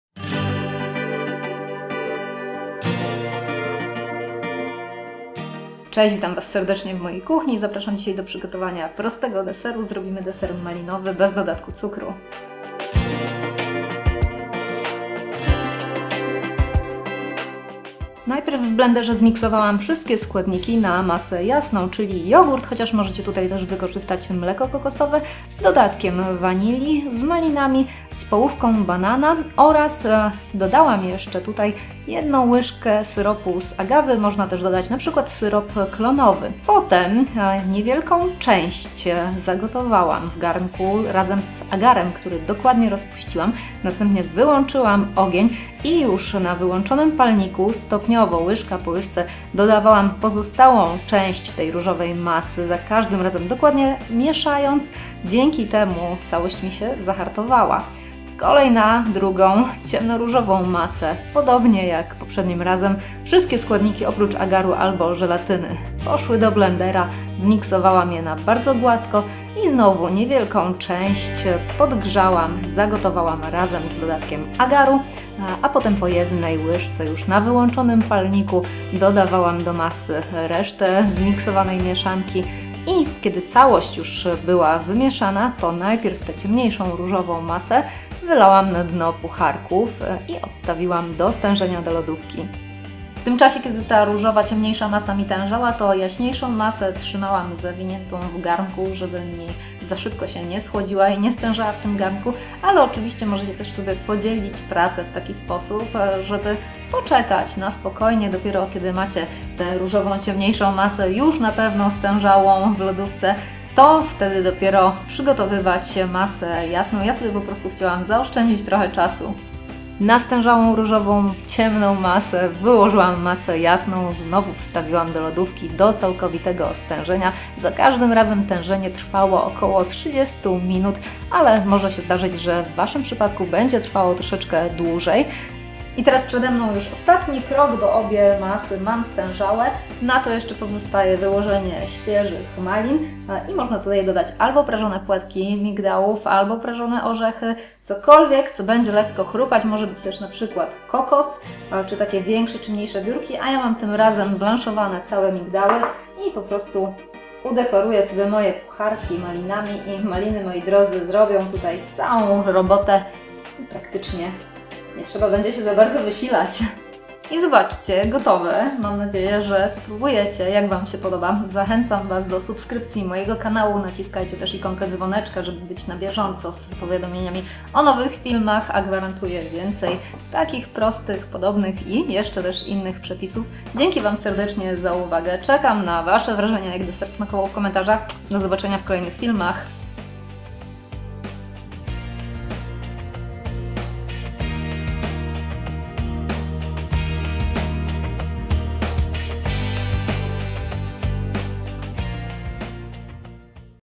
Deser z malinami 🍧 bez cukru i z opcją wegańską Ⓥ | Dorota Kamińska
Prosty do przygotowania deser z malinami na mleku kokosowym lub jogurcie naturalnym, z dodatkiem agaru lub żelatyny. To efektowny deser z malin z prostą dekoracją, serwowany w pucharkach.

Deser z malinami - składniki:

Warstwa ciemnoróżowa:
- 1 szklanka malin
- ½ banana (bardzo dojrzały)
- 1 łyżeczka agaru (lub 2 łyżeczki żelatyny)

Warstwa jasnoróżowa:
- ½ szklanki malin
- 1 szklanka jogurtu naturalnego (lub mleka
   kokosowego)
- ½ banana (bardzo dojrzały)
- 1 łyżeczka ekstraktu z wanilii
- 2 łyżeczki agaru (lub 4 łyżeczki żelatyny)

Do dekoracji:
- 1 szklanka malin
- 2 garście blanszowanych migdałów
- świeża mięta

Szczegółowy przepis krok po kroku z moim komentarzem i wersja do druku